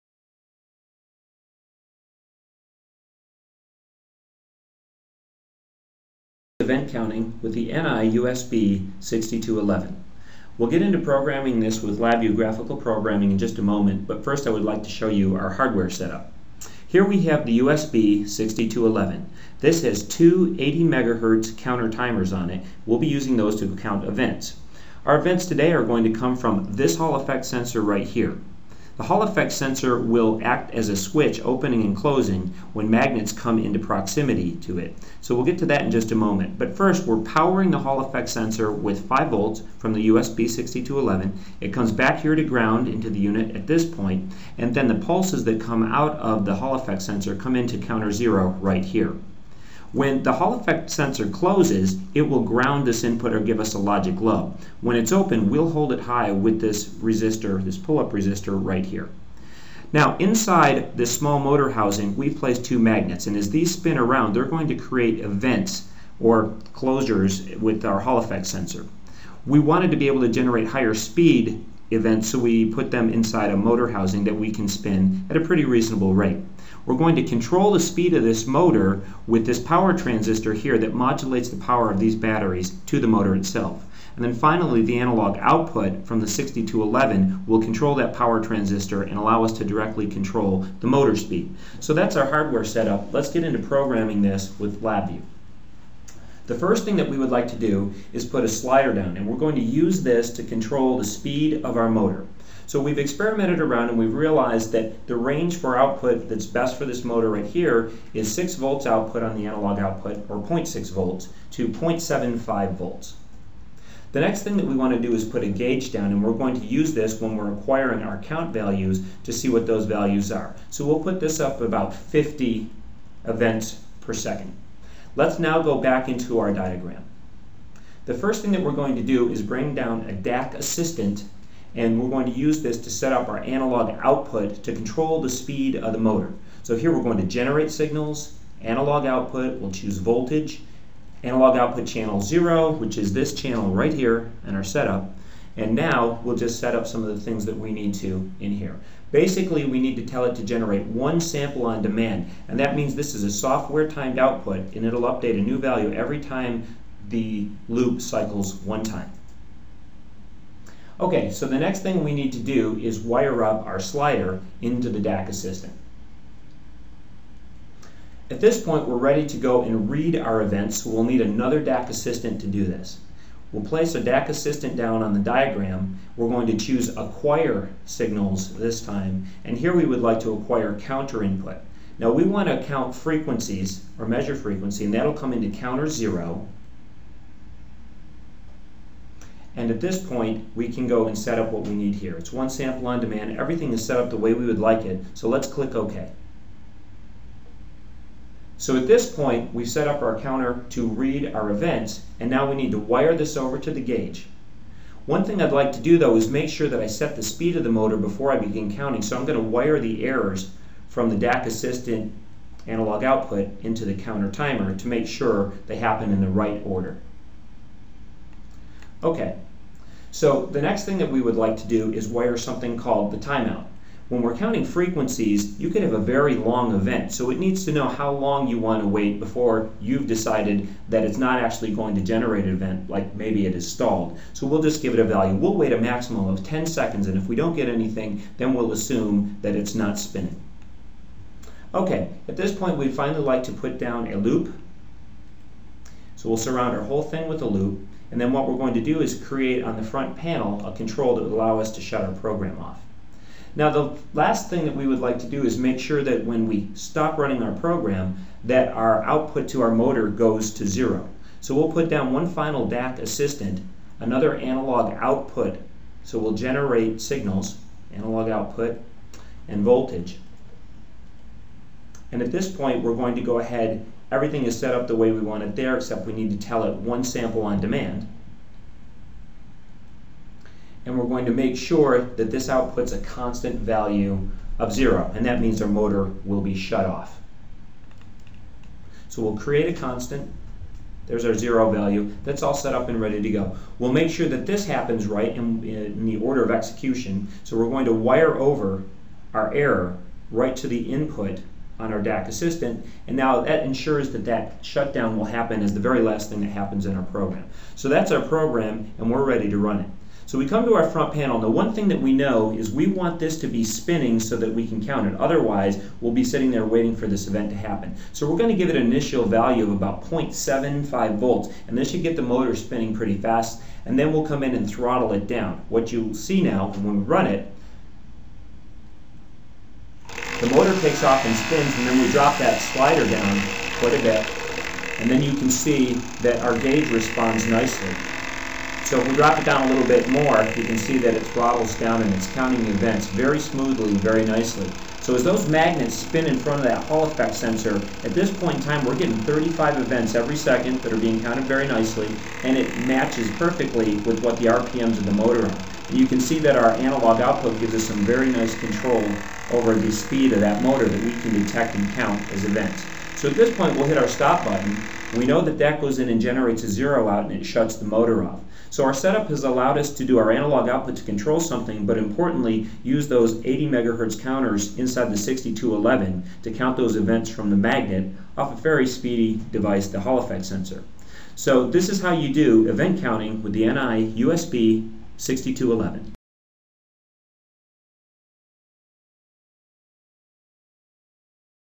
Event Count with USB-6211
This video demonstrates Triggered Analog Input and logging functionality of the NI USB-6211. This VI acquires analog input voltages coming from a potentiometer and uses a switch connected to digital input line to trigger the logging to file and show the saved data in a graph in LabVIEW